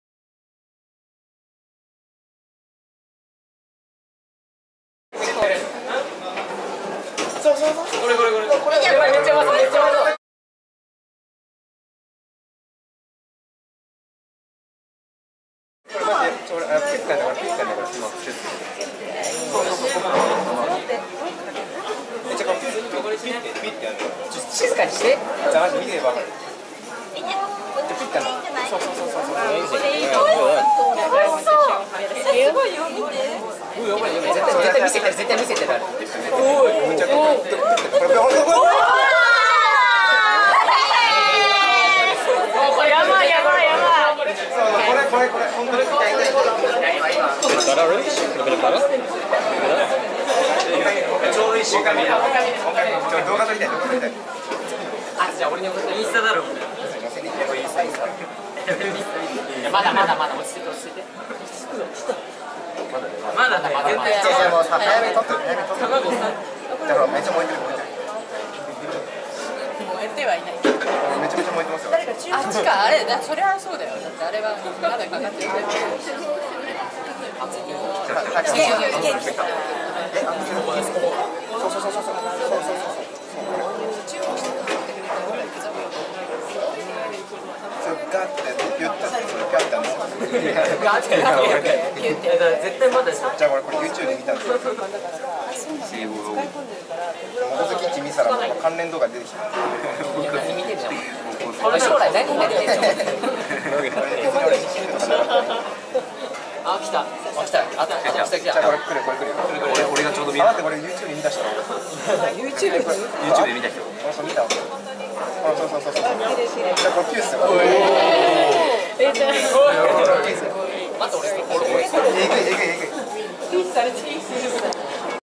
Seeing an awesome Omurice demonstration - Yokohama Tachibanatei
Awesome video of a pro Japanese omurice pan flipper. The chef is able to land the omelette from the pan onto the bed of rice each time.

This video shows two omelette flips through the shop's window. The chef is really cool.

We visited Yokohama's Red Brickhouse on 9 May 2017 and came across this store.

We got to see through the store's window during the lunch session at around 12:30 pm.